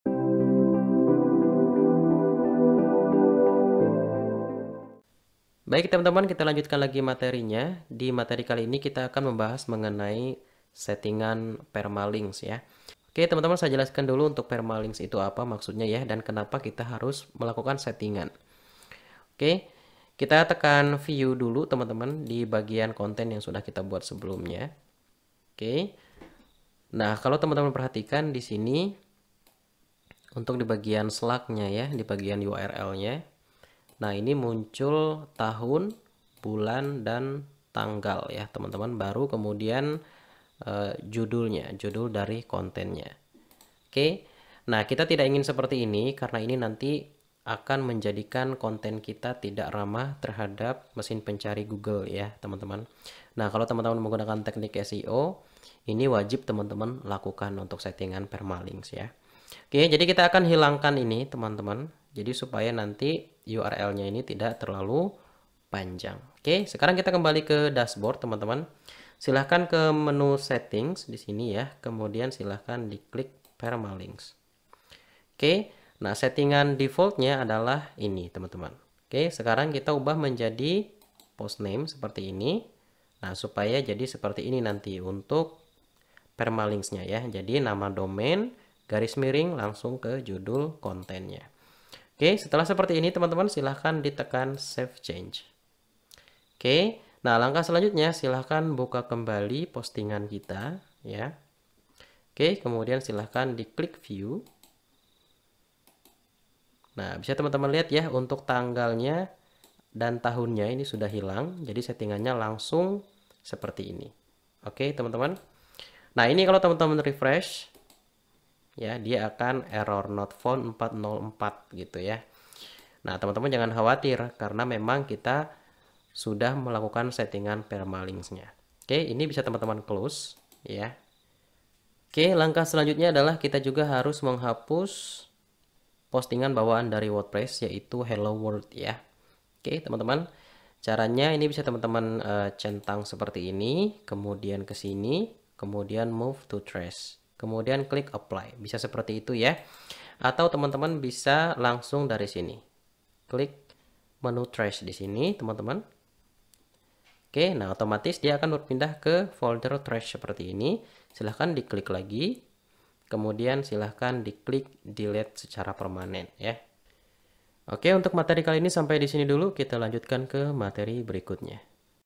Cara Setting Permalinks - Belajar Wordpress untuk Pemula Materi 2.8
Materi 2.8 Cara Setting Permalinks - Belajar Wordpress untuk Pemula, Video ini merupakan Bagian materi dari kelas online Gratis,
Belajar Wordpress untuk Pemula.